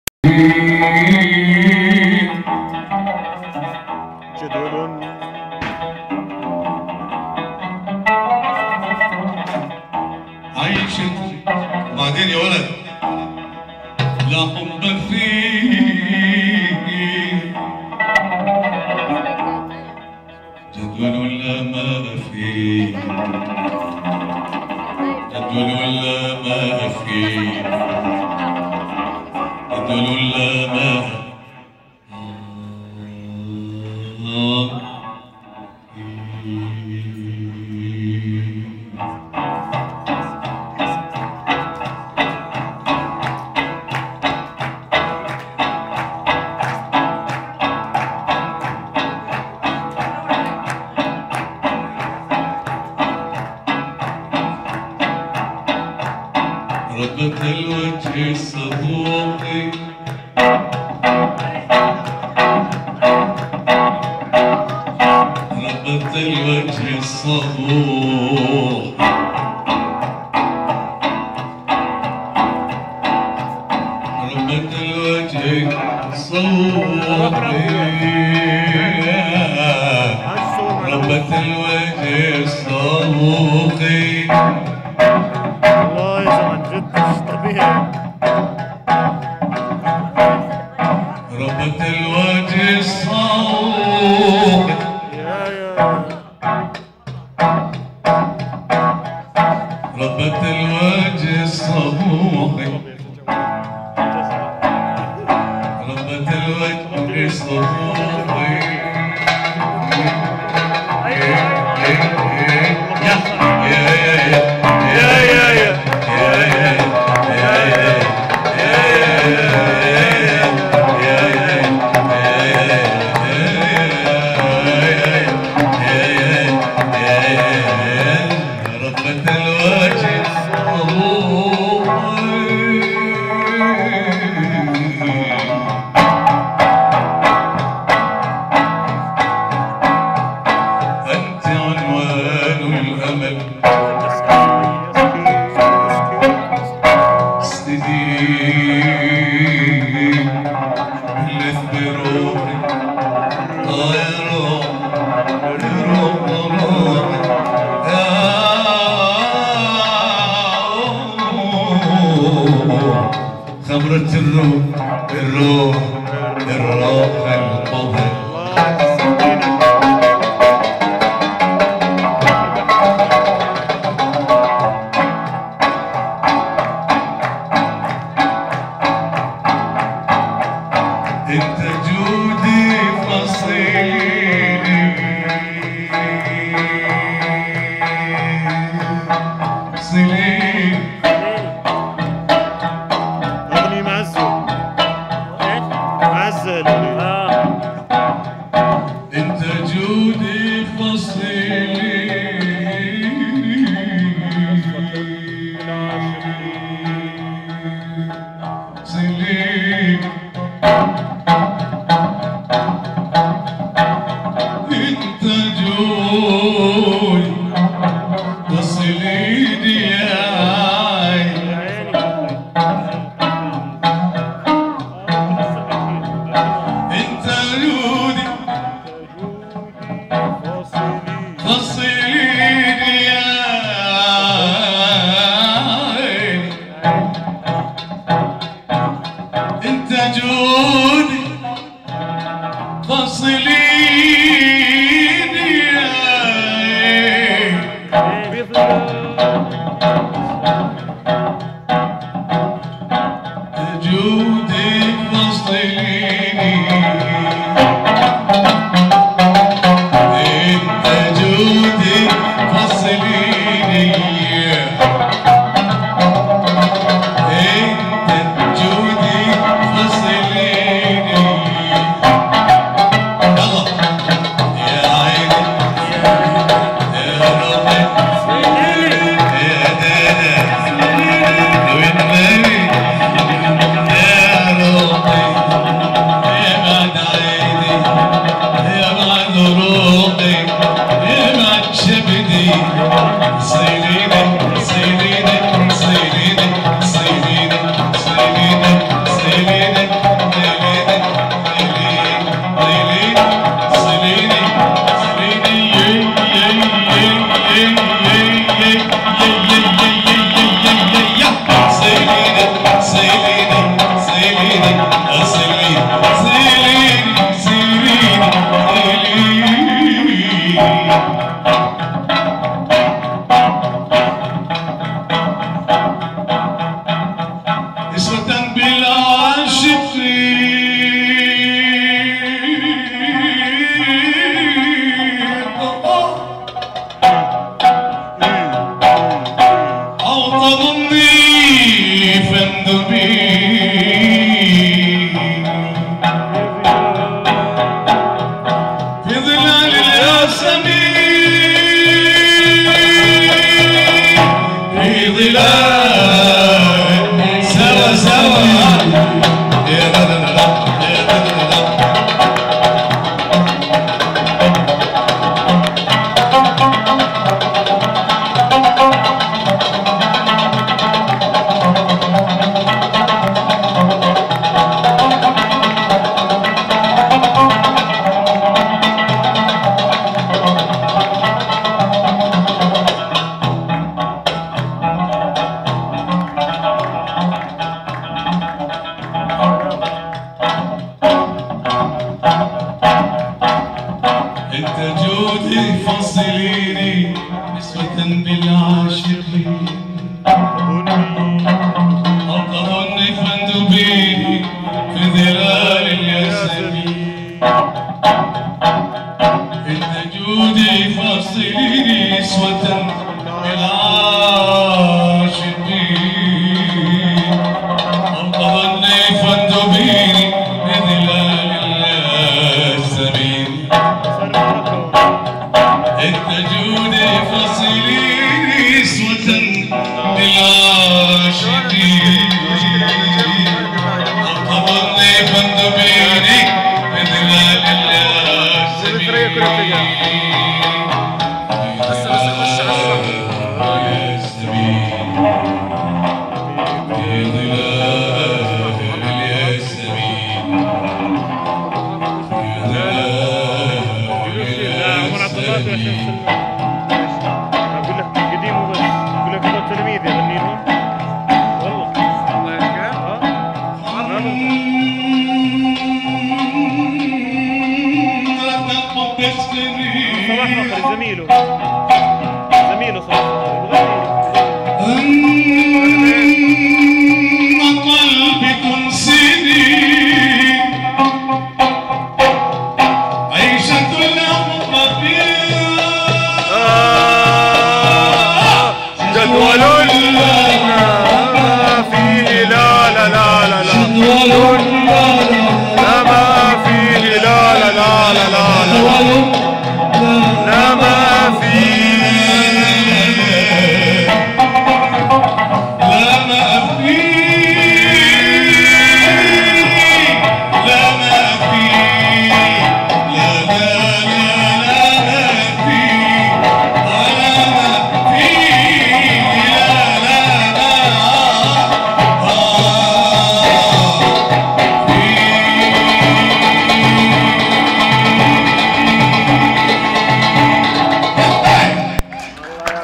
Sexy fannan
الفنان الأردني القدير اسماعيل خضر يغني عددا من الأغاني الكلاسيكية في عرس أنس فتحي العمايره - أفراح عمايره عربيات يوم 16/10/2009 في فندق الفنار عمان الأردن
Jordanian famous artist singer Ismail Khader singing classic songs at Anas Al Amaireh wedding in Oct. 16, 2009 in Alfanar Hotel Amman Jordan - Amaireh Arabeyyat Wedding